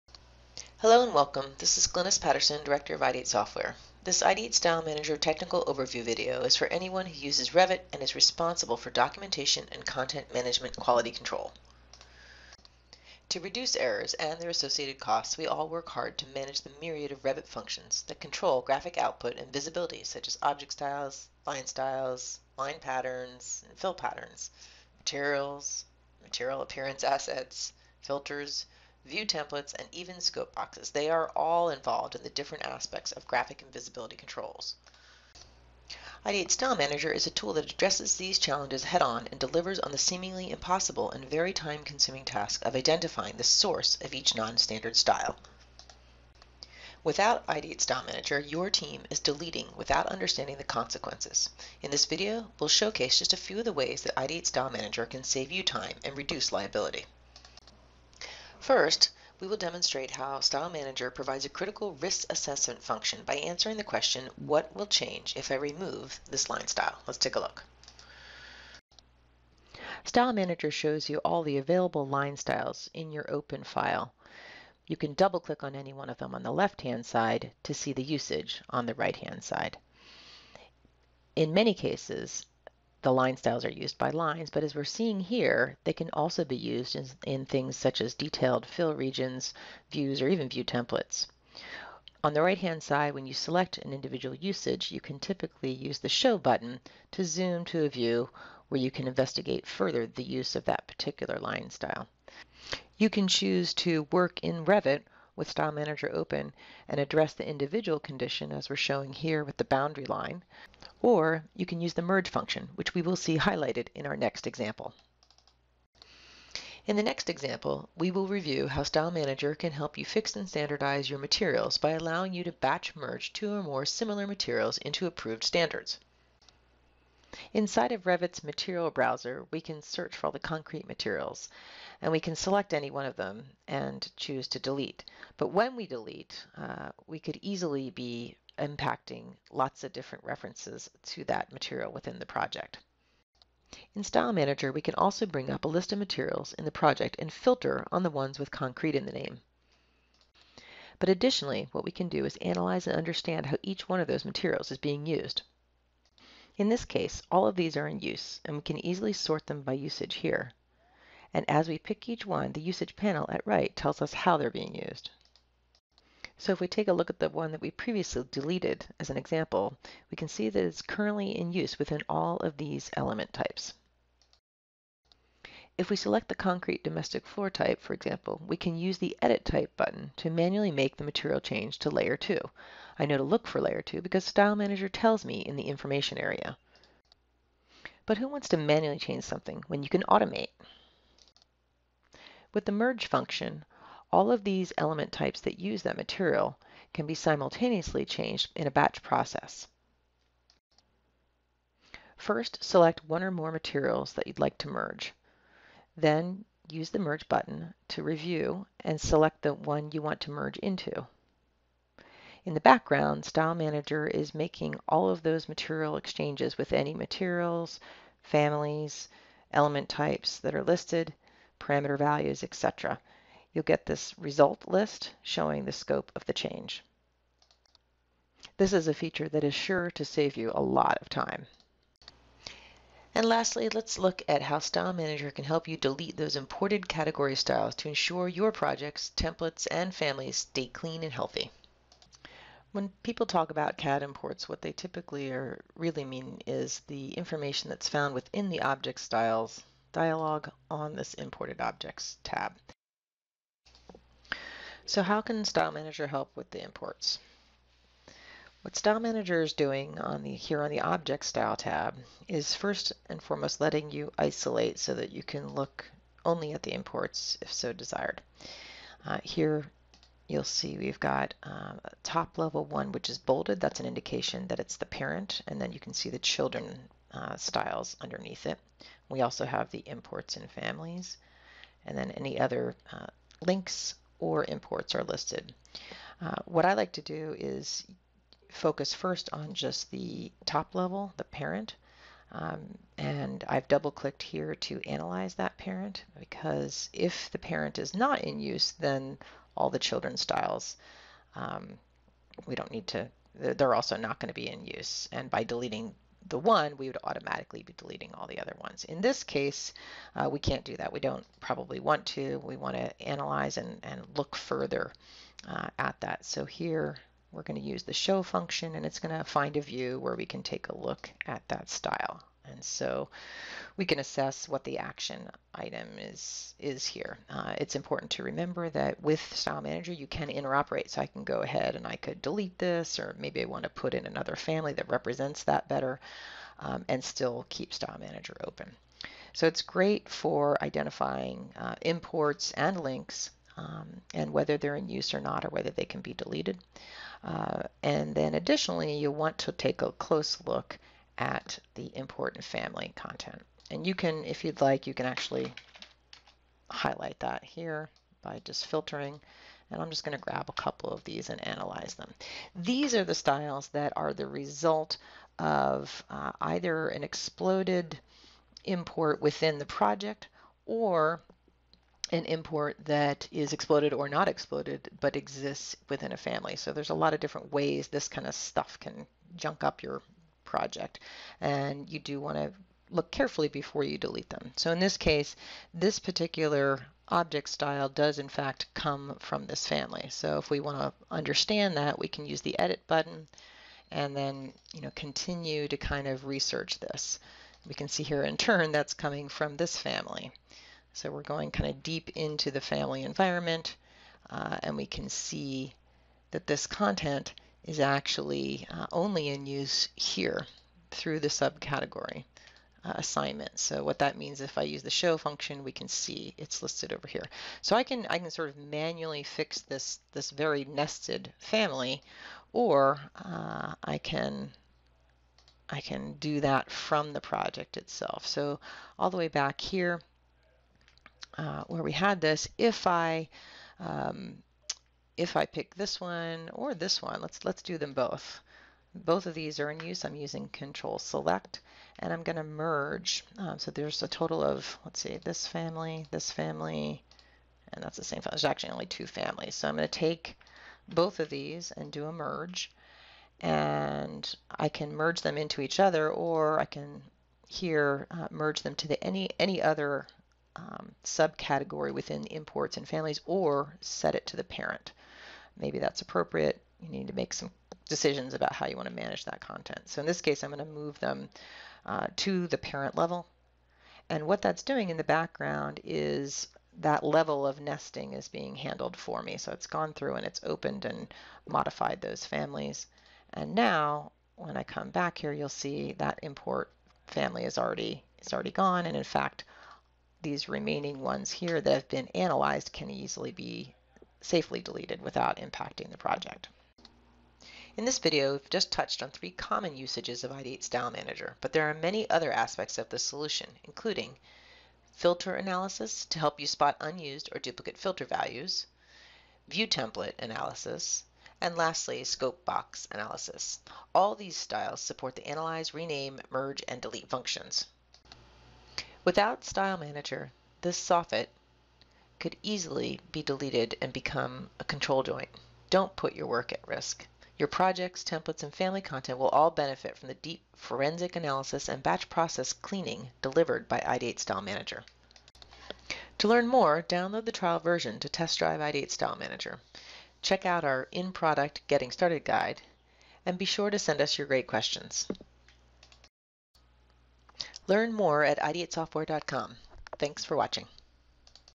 Introduction to Ideate StyleManager for Revit
Ideate StyleManager for Revit is a solution developed by Ideate Software that lets you analyze, rename, merge and delete Revit object styles, line styles, line patterns, fill patterns, materials, material appearance assets, filters, view templates and scope boxes. Without StyleManager your team is deleting without understanding the consequences. In this video we will showcase three examples of how Ideate StyleManager can save you time and reduce liability:

• Risk Assessment of style deletion
• Batch merging materials
• Delete CAD Import Styles

Restore your project, template and your Revit family content to approved BIM standards with Ideate StyleManager to ensure fewer last-minute printing challenges to reduce liability due to visibility and graphic style issues.